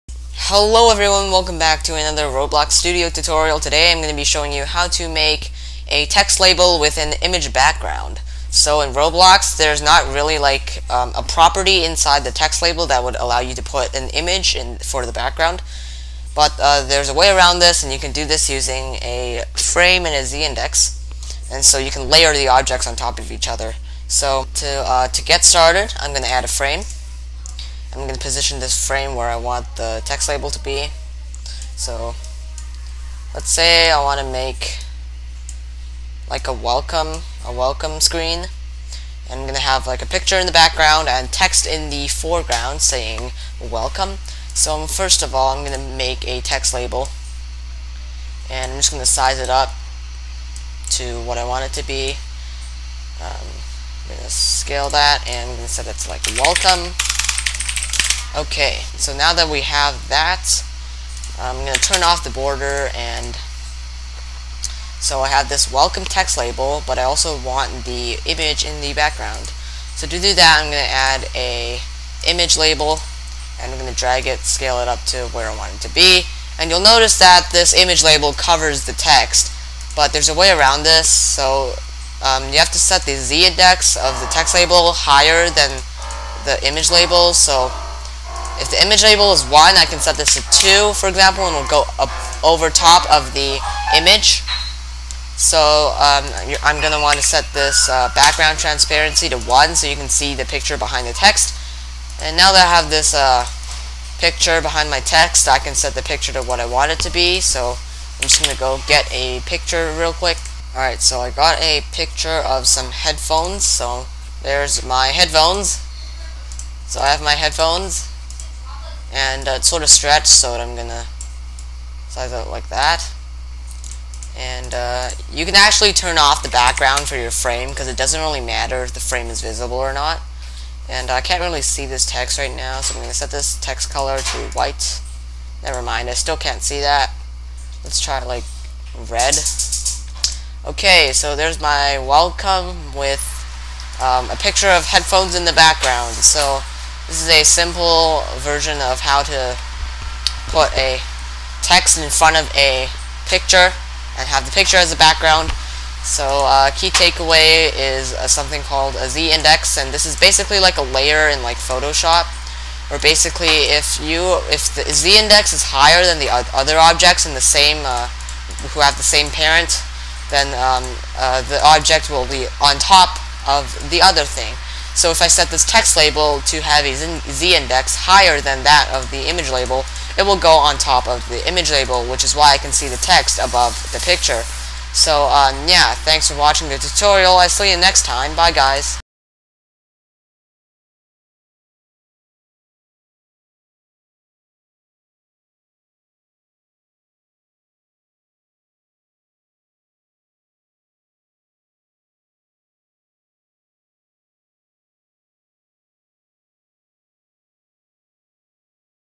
Text With Image Background In Roblox Studio! Roblox Basic GUI Tutorial
Learn how to make a TextLabel with an image background using the Z-Index property available in GUI objects.
Z-Index is a property that controls the layer on which GUI objects sit. This is sort of like in image editing software, where if an object is on a higher layer, it will appear on top of other objects below it in the layers.